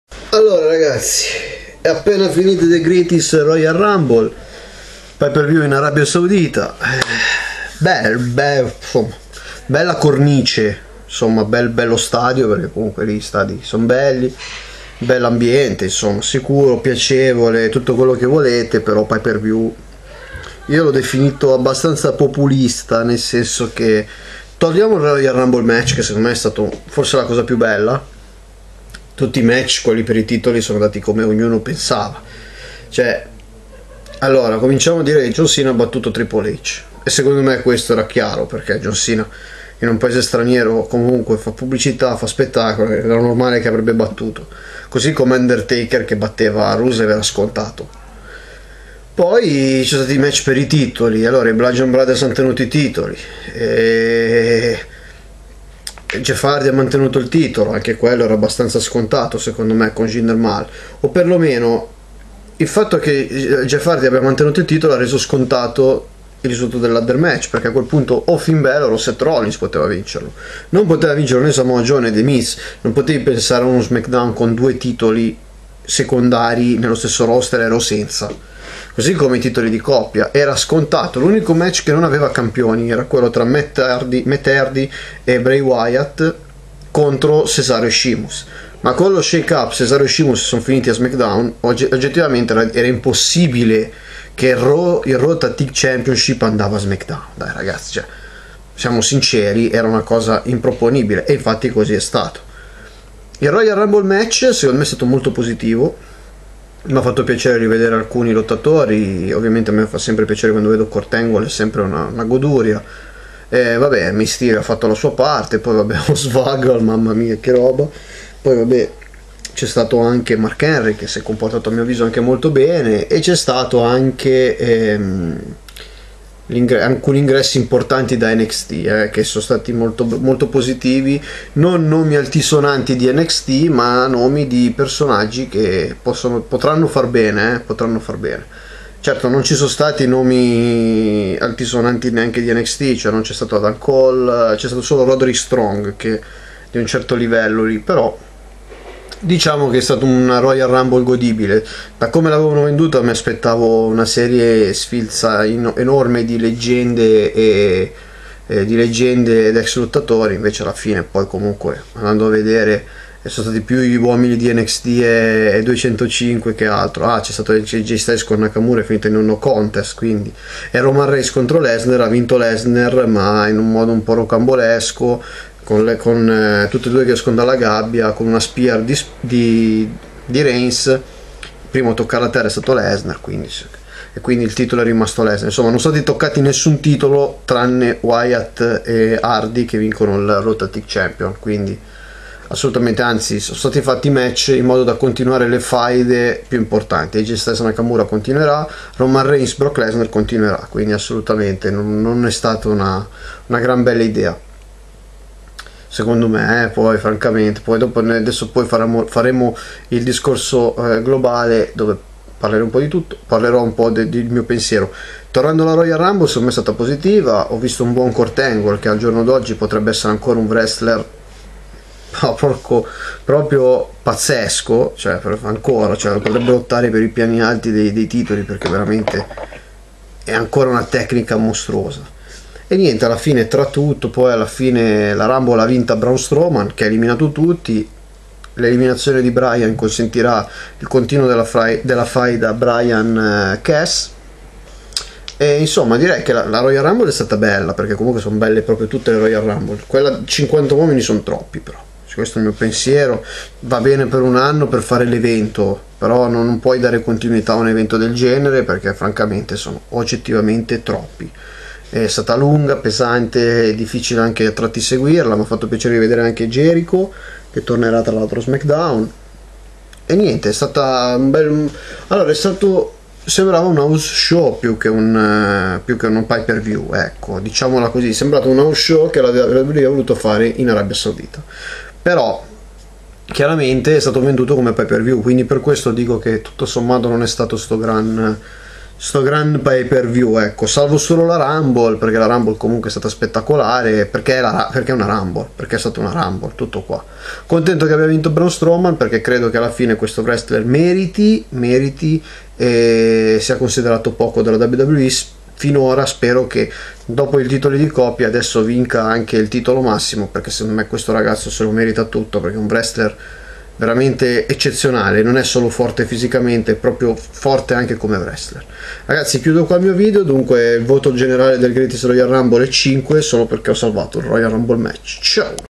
WWE Greatest Royal Rumble
appena terminata Greatest royal rumble ecco i miei commenti sullo show wwe